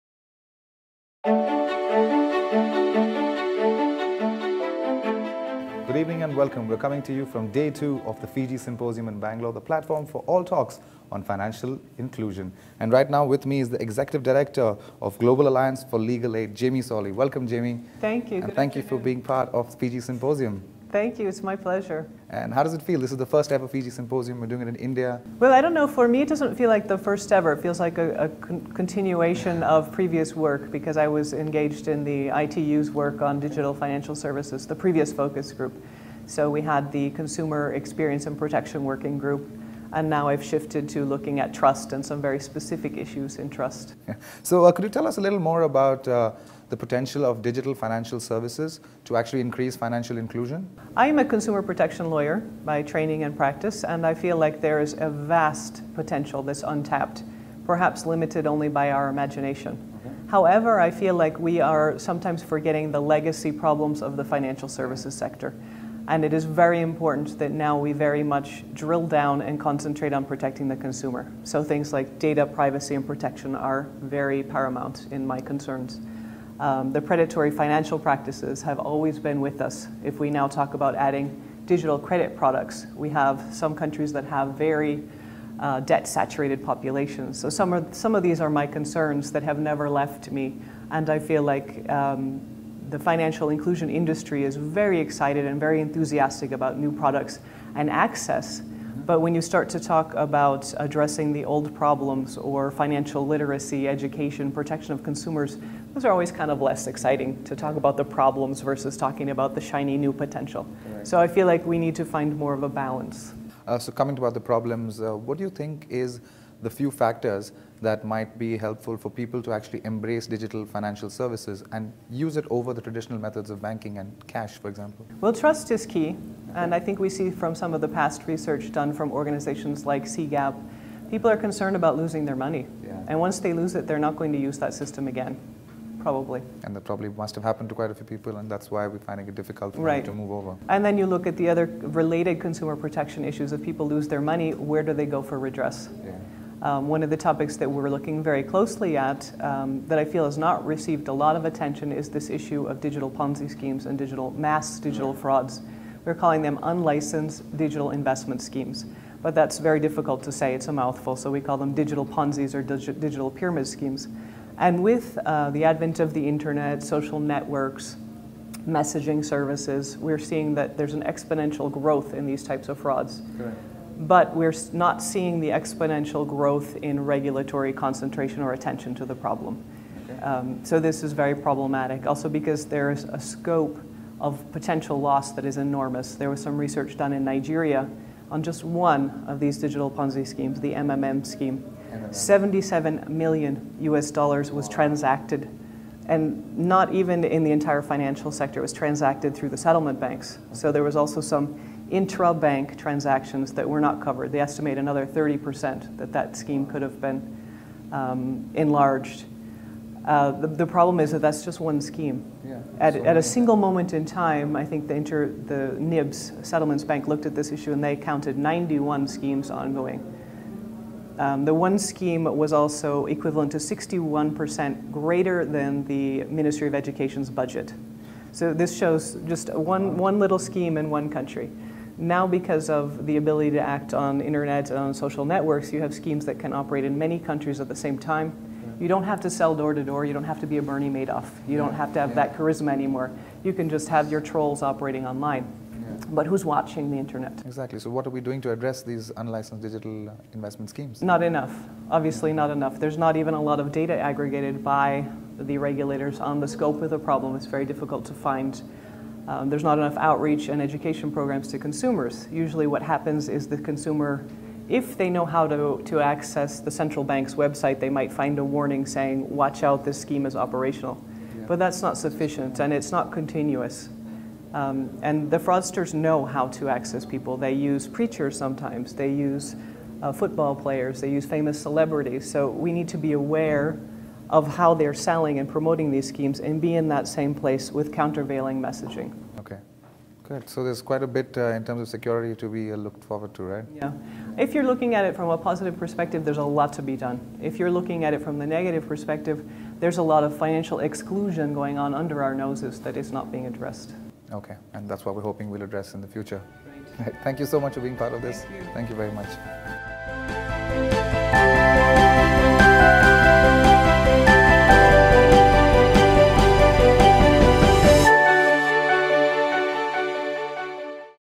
ITU INTERVIEWS @ FIGI Symposium 2017: Jami Solli, Executive Director Center for Legal Aid
Jami Solli, Executive Director, Center for Legal Aid, discusses consumer protection issues related to digital finance.